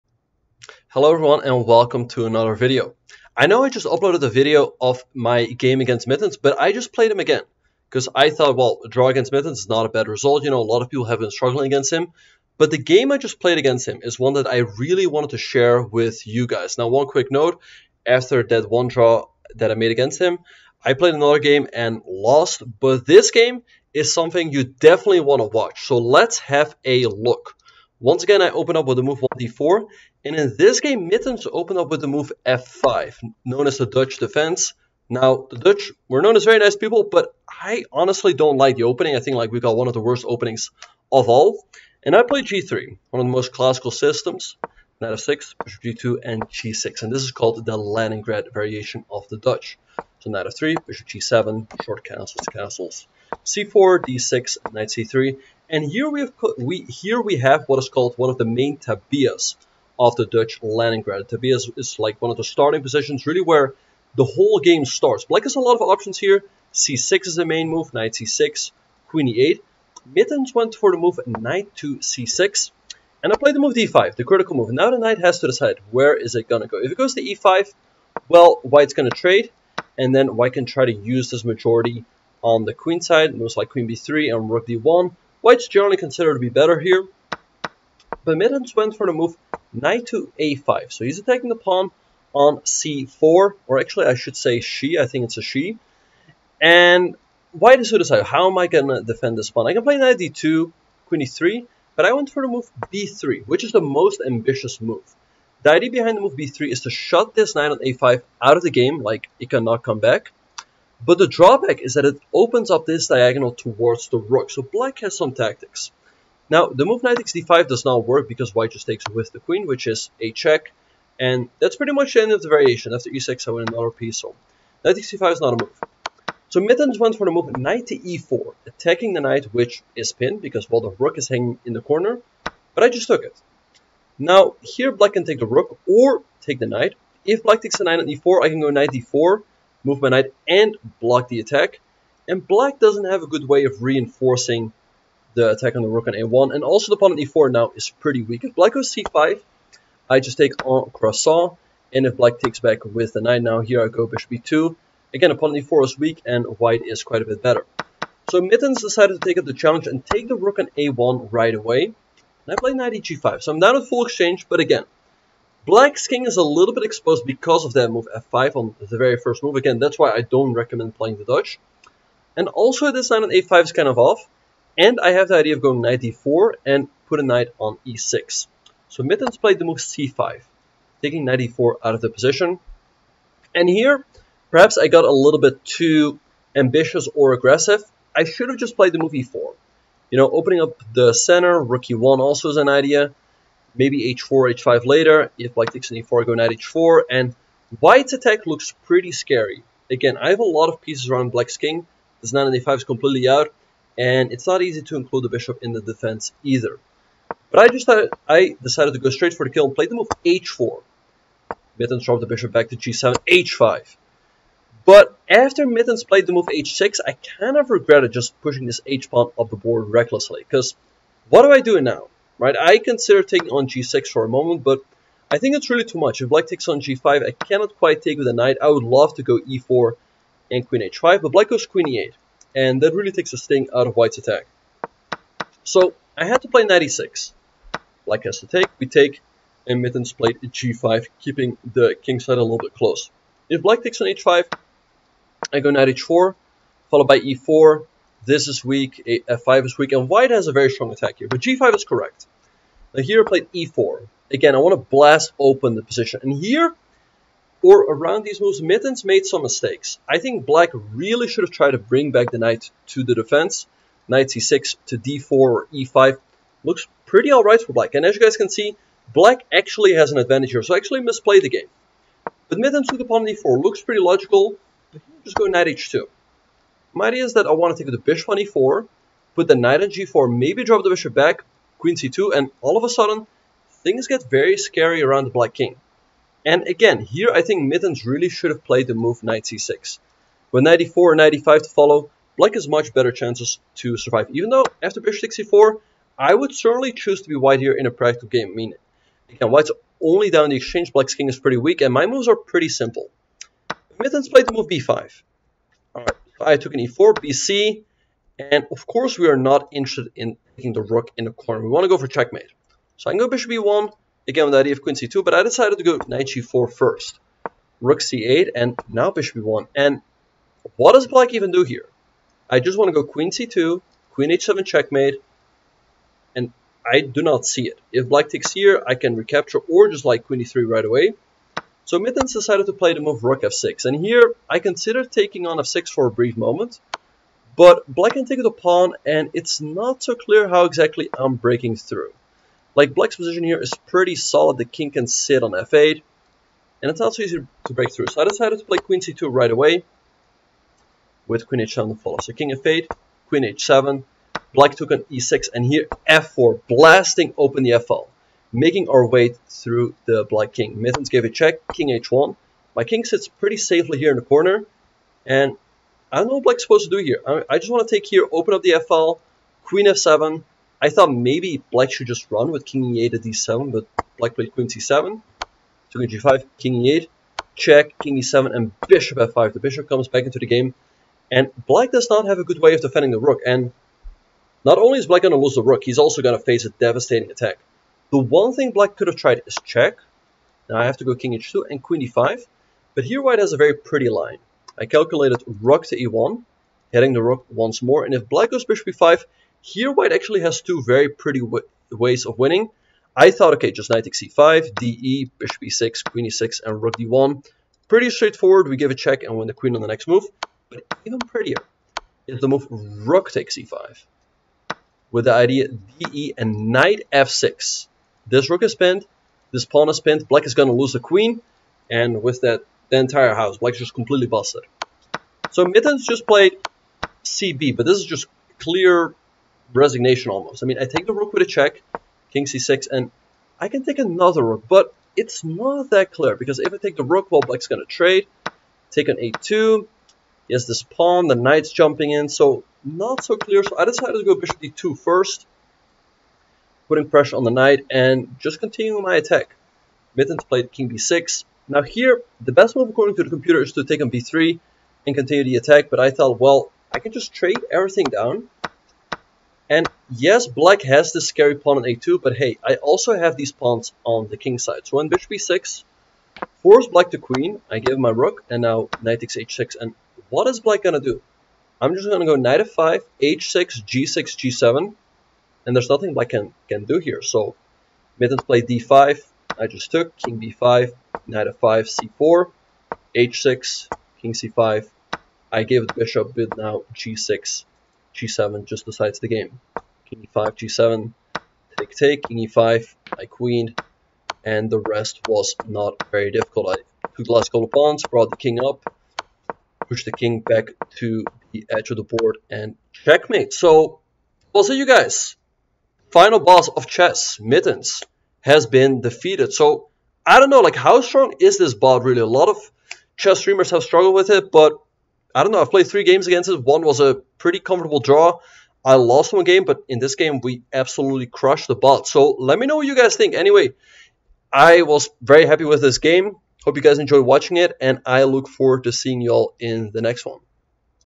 I Absolutely DESTROYED Mittens
Chess Grandmaster Benjamin Bok takes on chess.com's newest bot, Mittens, again. Many chess content creators have played Mittens, including Hikaru Nakamura (GMHikaru), Levy Rozman (GothamChess), and Eric Rosen.